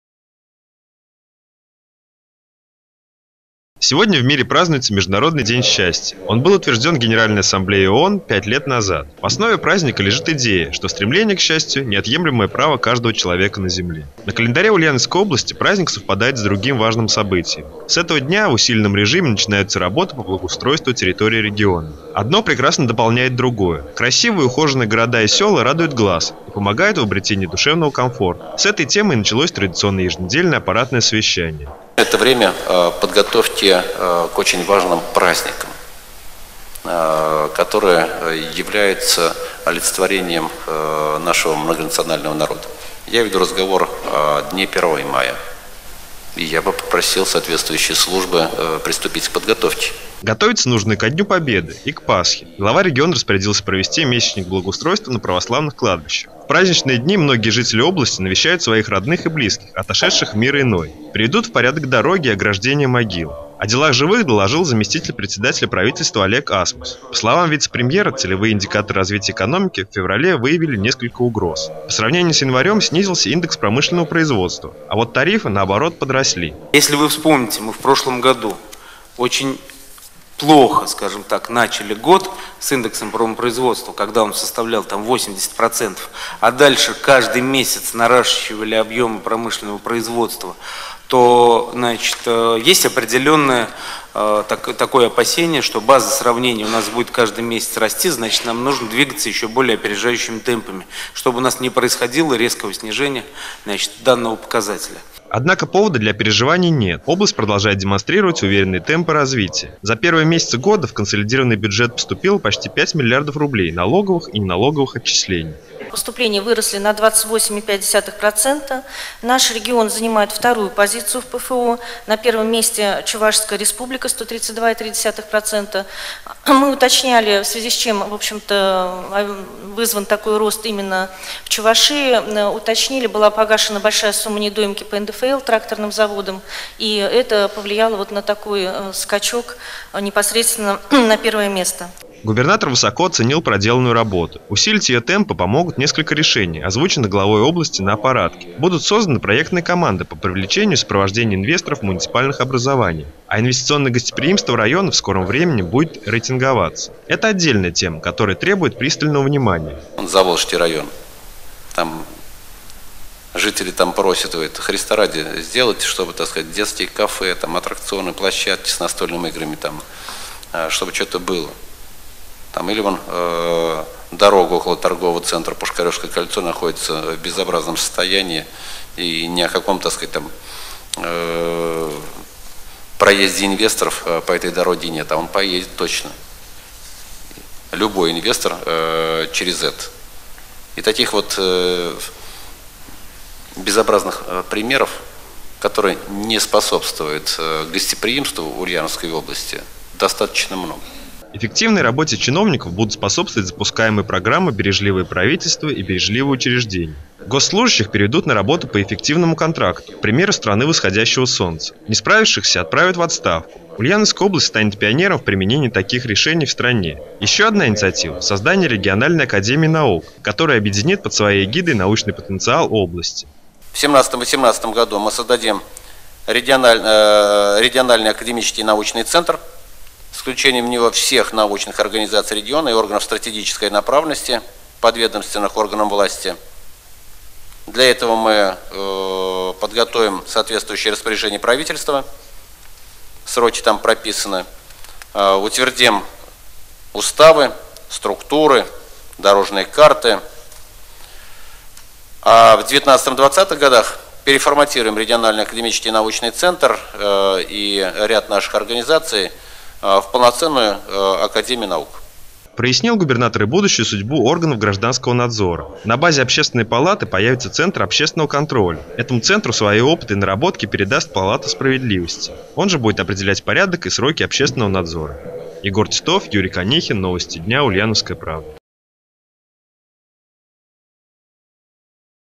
Аппаратное совещание Губернатора от 20.03.2017
Совещание было посвящено благоустройству территорий региона, подготовке ко Дню Победы и к Пасхе. Губернатор озвучил и ряд инициатив.